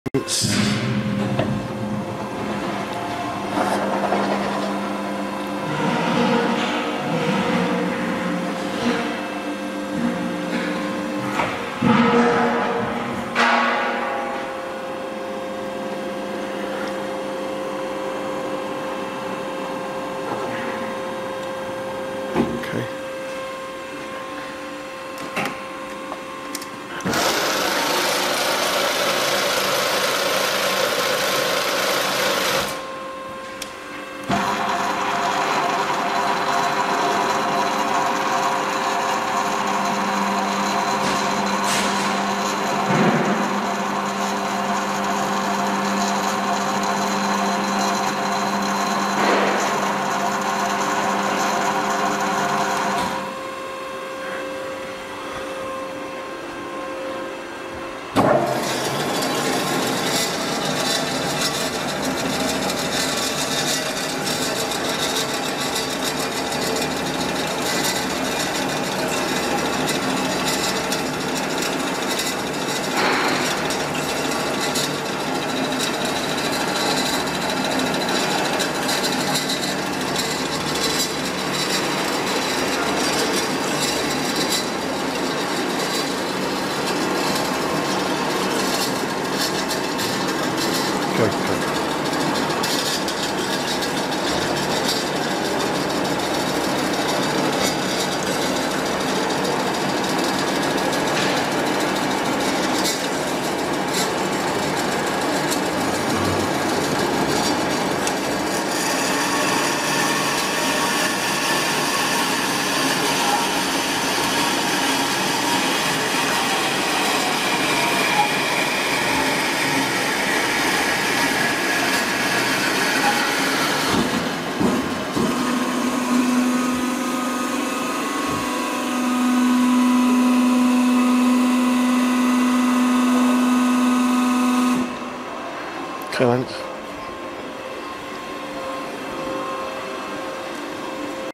Kasto PBA 320/460 AV Automatic Horizontal Bandsaw
Capacity 460mm Diameter x 200mm Long or 320mm
Diameter x Unlimited Length
Input 415/3/50 ac
Speeds 8-82 Metres per Minute
Vice opens 500mm
Stroke of feed vice 580mm
Multi Feed Yes
Fitted with Coolant & Variable Downfeed
Length 9' / 2.74m
Width 7'6" / 2.29m
Height 5'4" / 1.6m
Nett weight approx. 2.1/2 ton / 2540 kgs